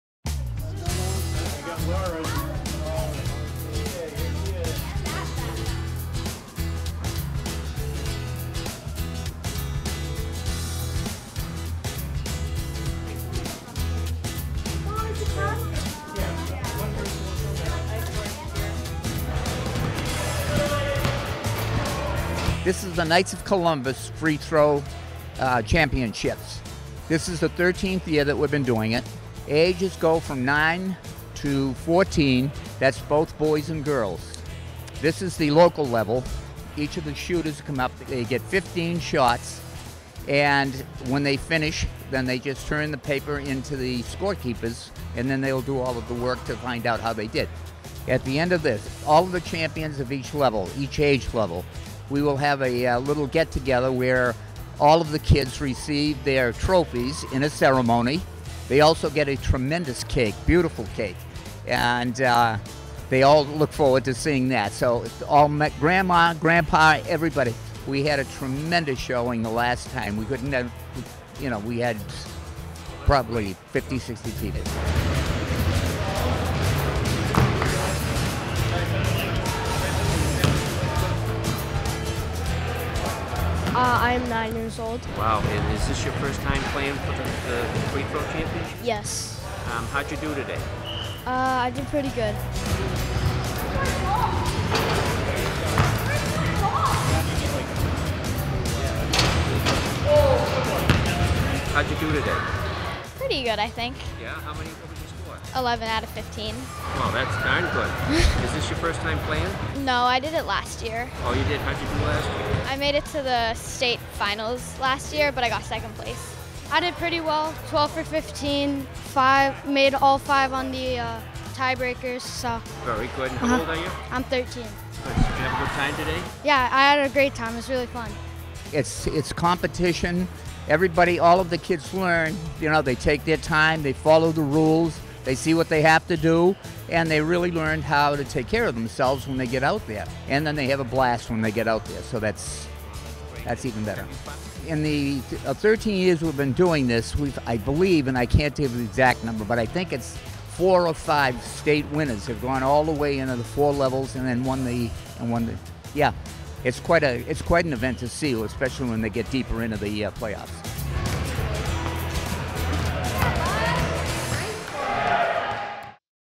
Knights of Columbus Free Throw Competition 2024
A look at the annual Knights of Columbus Free Throw Competition on January 26, 2024.